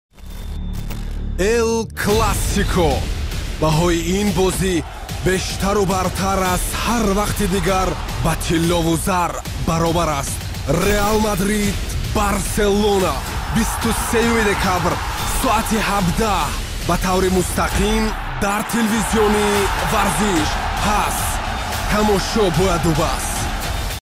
Эл – Класико 23/12/2017, 17:00
Эл – Класико
Баҳои ин бозӣ бештару бартар аз ҳар вақти дигар ба тиллову зар баробар аст.
“Реал Мадрид” – “Барселона”
23-юми Декабр
Соати 17:00 бо вақти Душанбе
Ба таври мустақим дар ТВ “Варзиш”